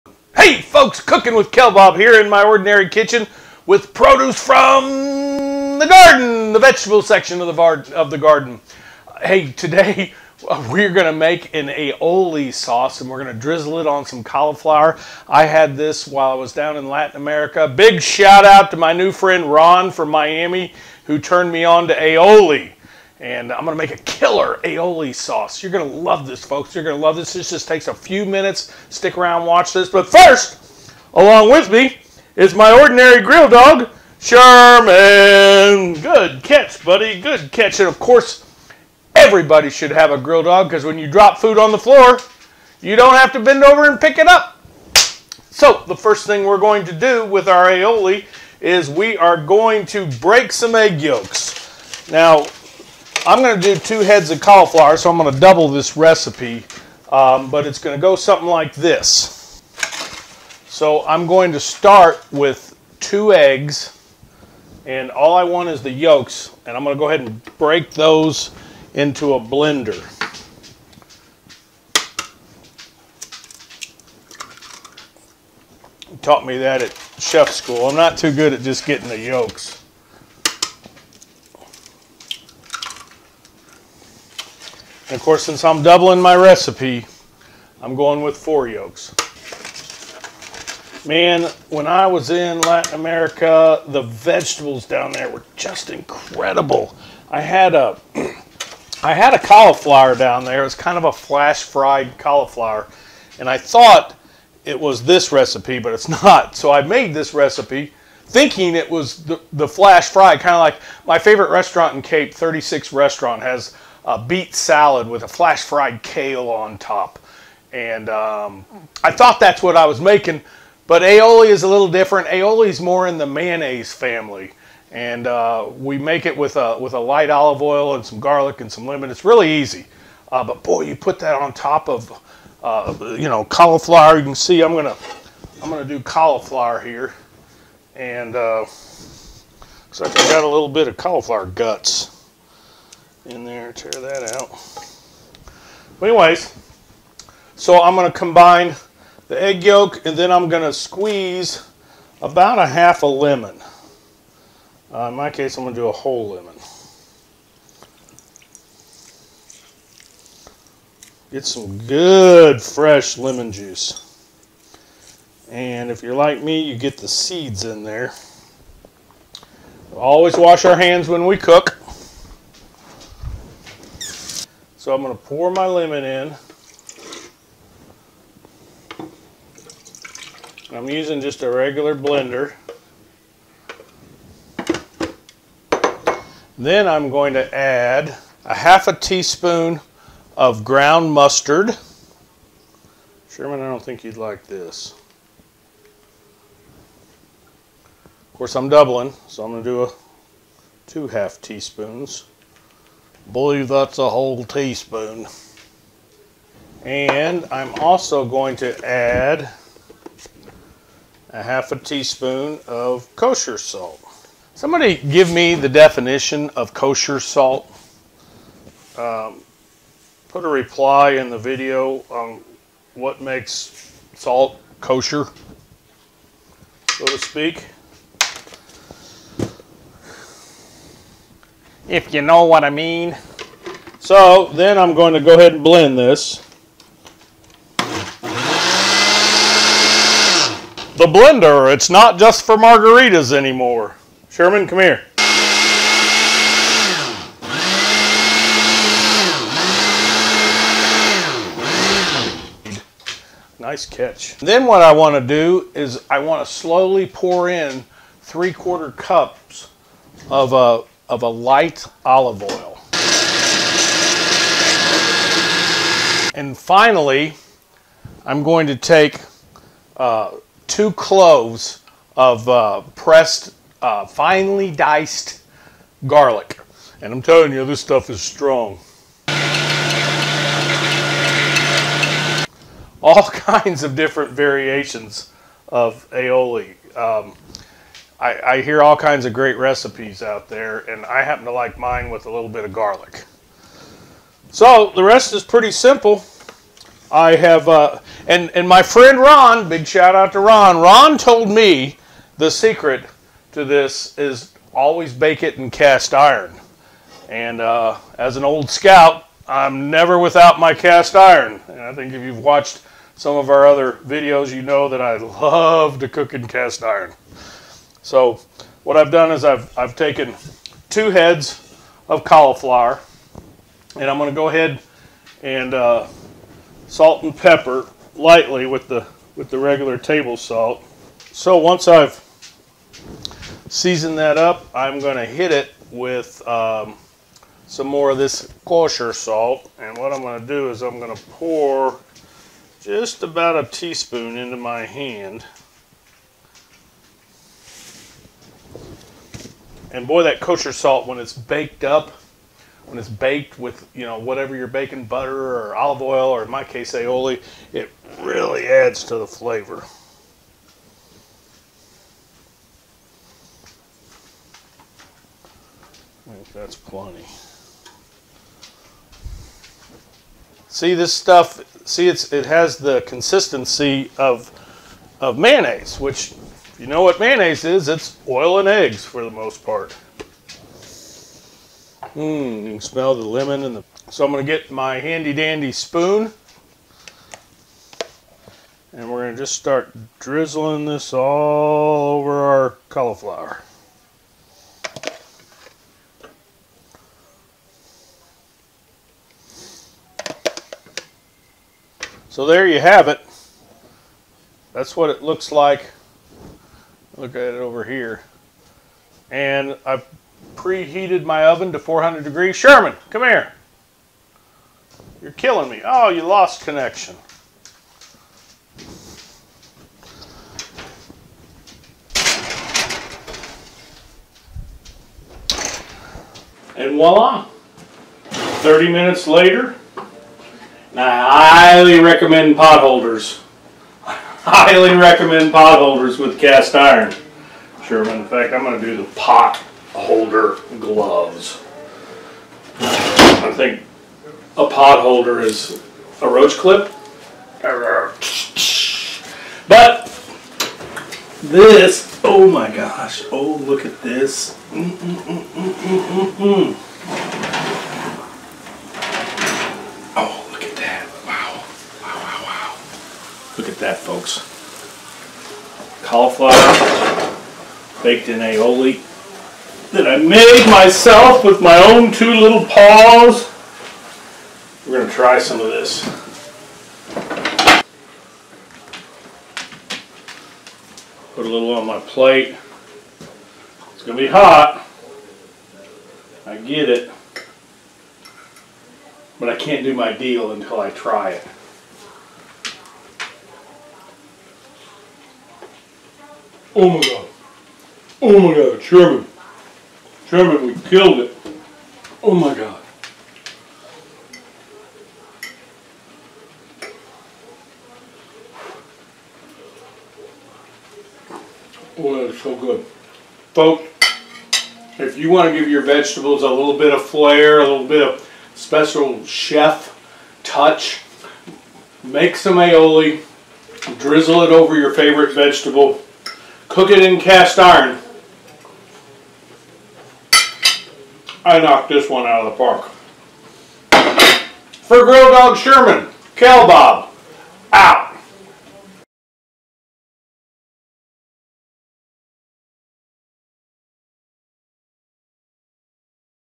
Roasted Cauliflower Aioli Recipe - Cooking with Kelbob
I ate some outstanding vegetables in Medelin Colombia and Santo Domingo Dominican Republic. My friend Ron mentioned that preparing a garlic based aioli sauce and roasting cauliflower in cast iron is a great recipe. This one is a keeper folks. You gotta try it. Kelbob Out